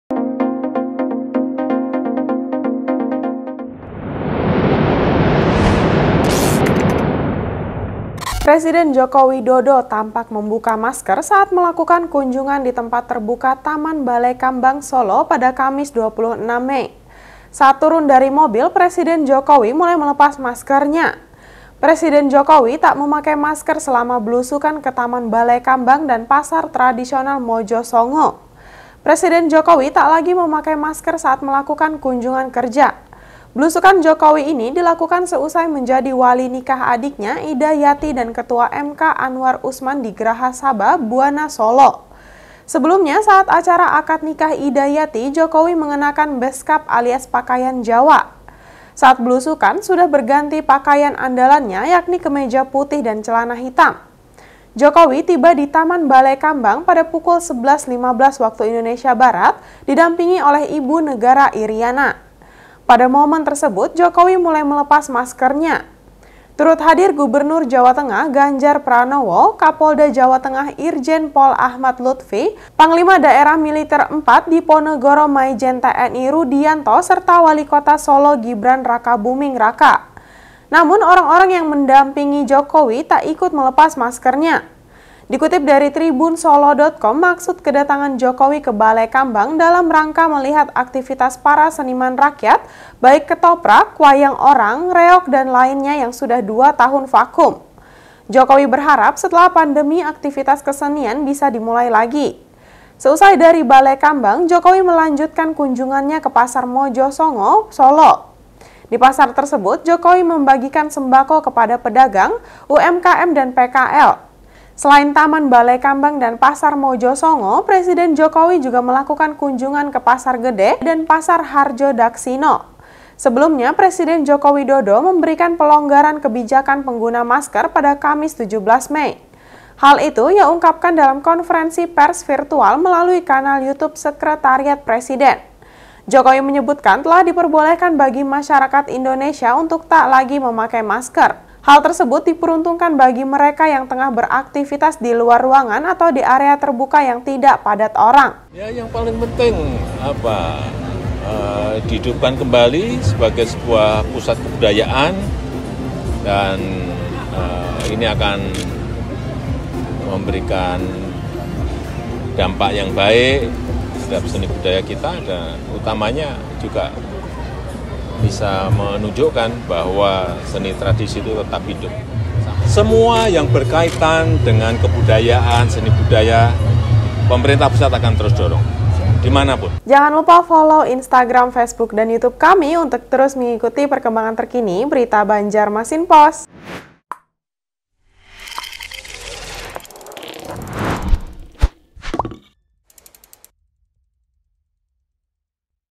Presiden Jokowi Tampak Lepas Masker saat Kunjungan Kerja di Taman Balekambang
BANJARMASINPOST.CO.ID - Presiden Joko Widodo (Jokowi) tampak membuka masker saat melakukan kunjungan di tempat terbuka Taman Balekambang Solo, Kamis (26/5/2022).

Saat turun dari mobil, Presiden Jokowi mulai melepas maskernya.

Presiden Jokowi tak memakai masker selama blusukan ke Taman Balekambang dan pasar tradisional Mojosongo.(Tribunnews.com)